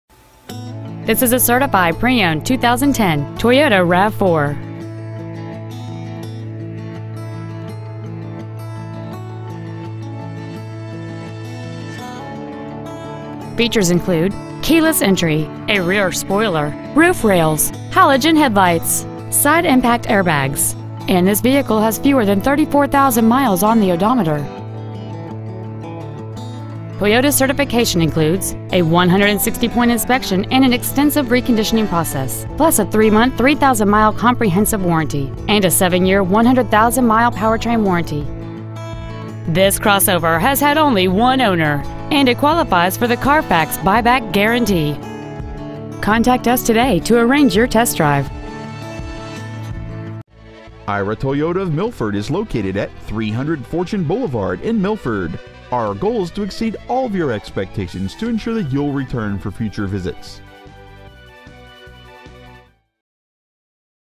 Pre-Owned 2010 Toyota Rav4 Milford MA
We have been honored to serve the Milford MA area , we promise that your experience at our dealership will exceed your expectations !

 Year : 2010
 Make : Toyota
 Model : Rav4
 Engine : 4 - CYL.
 Trans . : AUTOMATIC
 Exterior : BLACK FOREST P
 Miles : 33,806
 Interior : OAK